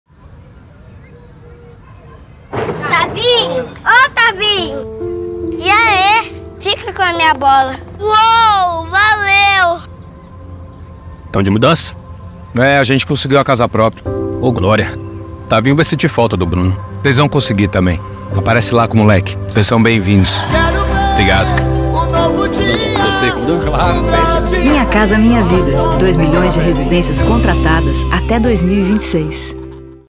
O Brasil é um só povo: Minha Casa, minha Vida
Em cada lar deste país, mora uma família diferente, com sonhos diferentes. Mas a conquista da casa própria é um sonho que une todas elas. Agora, mais brasileiros podem realizar esse sonho com o Minha Casa, Minha Vida. Realizar sonhos nos une. Compartilhe OBrasilEUmSoPovo "